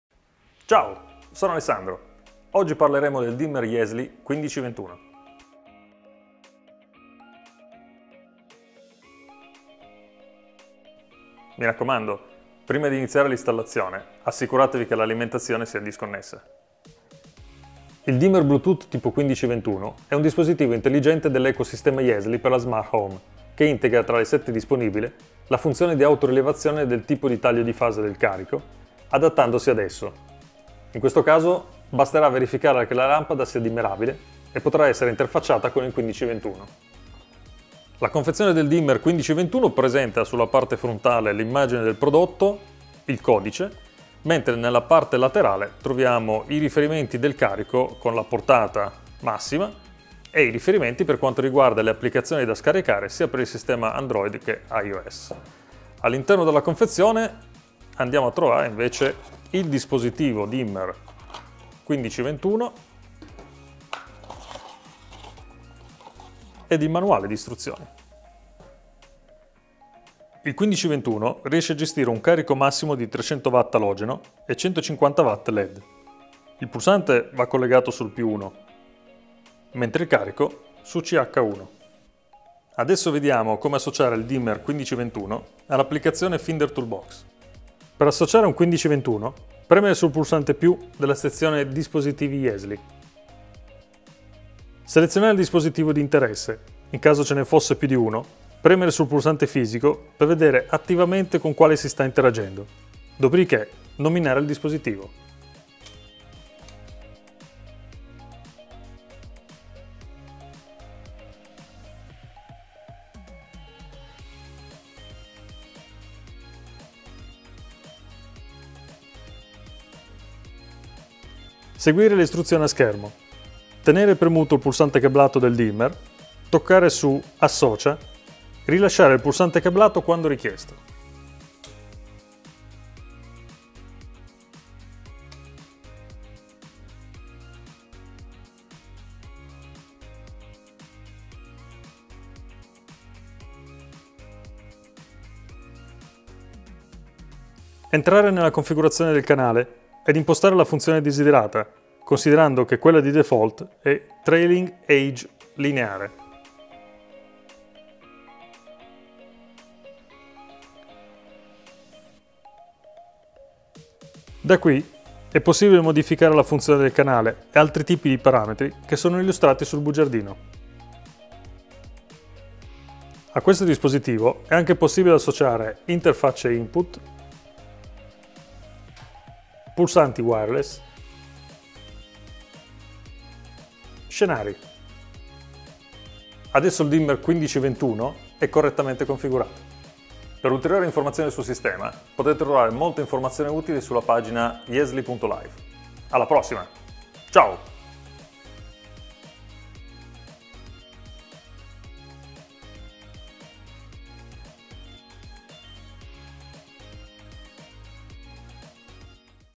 YESLY | Dimmer | Type 15.21
The 15.21 Bluetooth dimmer is a smart device that's part of the YESLY system.